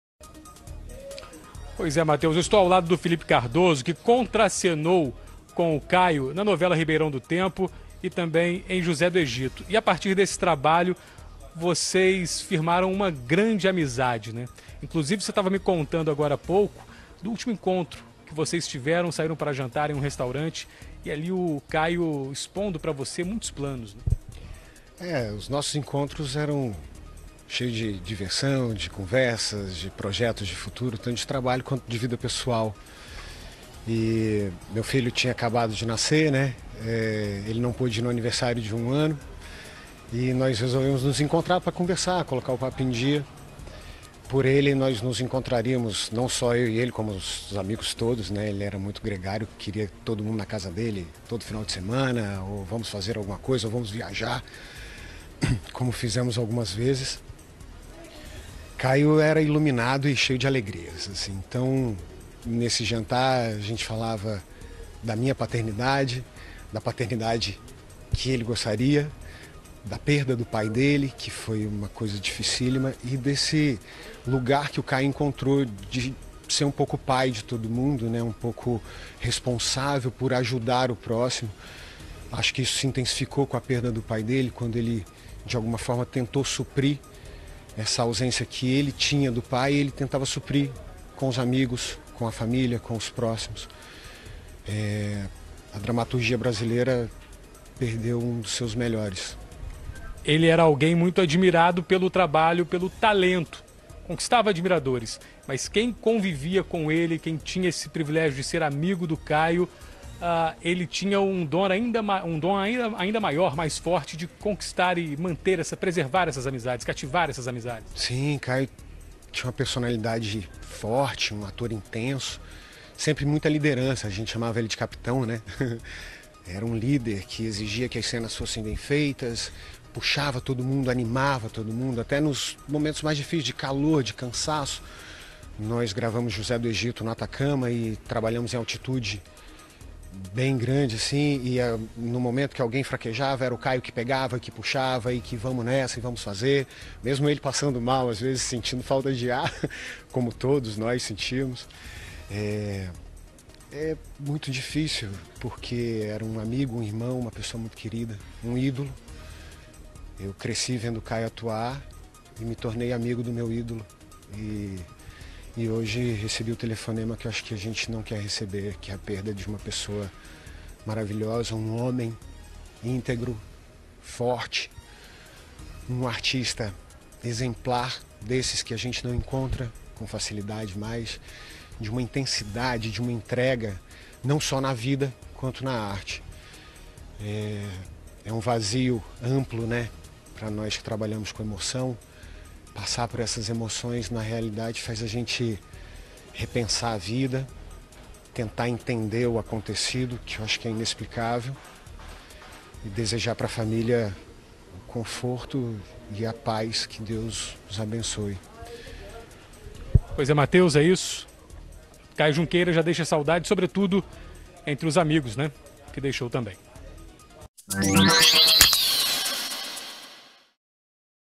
Famosos relembram a carreira do ator Caio Junqueira
Amigos e colegas de elenco prestam as últimas homenagens ao ator Caio Junqueira, que morreu nesta quarta-feira (23), após uma parada cardiorrespiratória. Para assistir ao conteúdo na íntegra, acesse PlayPlus.com